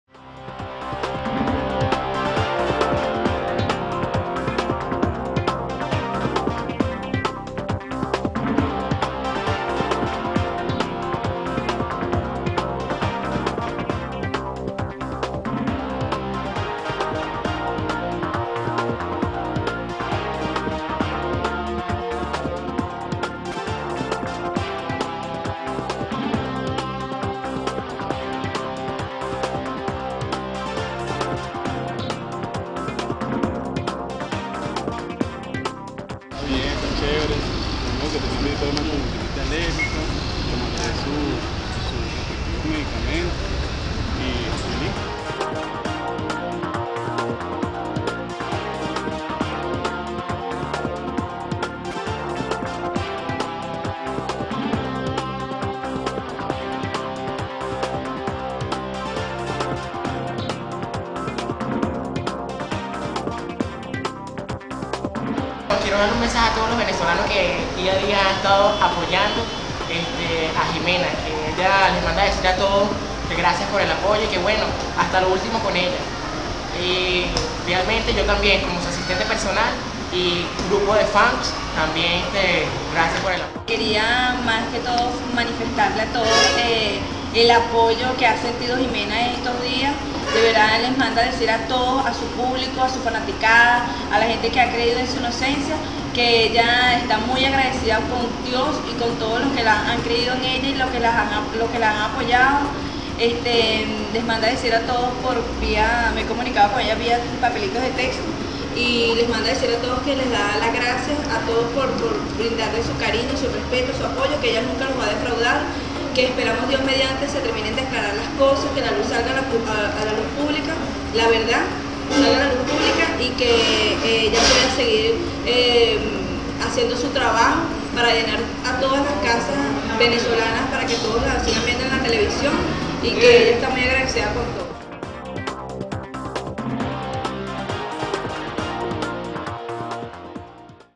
Situación de "Rosita" en la Estación Policial San Carlos
Jimena Araya, conocida en el mundo artístico como "Rosita" está detenida desde el martes en un calabozo de sólo 3 metros cuadrados, en la Estación Policial San Carlos, de Maracay estado Aragua. Todo el ambiente es de color blanco, tiene cuatro camas de cemento y no hay ventanas, sólo los barrotes.